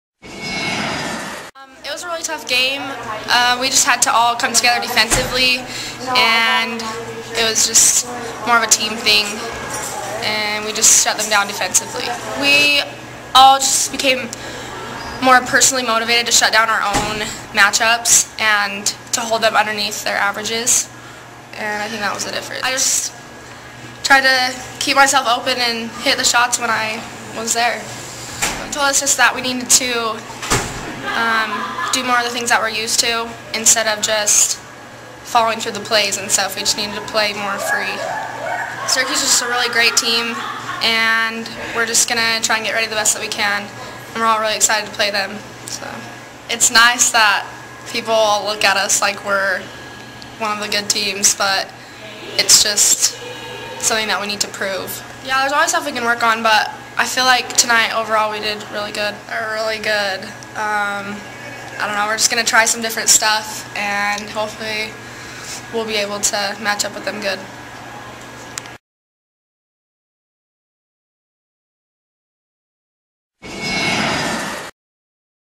High school girls basketball: Maddi Smith (Layton Lancers) post-game interview 02-10-12.
Maddi Smith (Layton Lancers) post-game interview 02-10-12. After the Layton Lancers vs. Davis Darts Utah high school girls basketball game, final score Layton 48 Davis 27. Smith finished with 8 points, and two 3-point buckets. The 5A Region 1 game was played at Davis High School, Kaysville, Utah.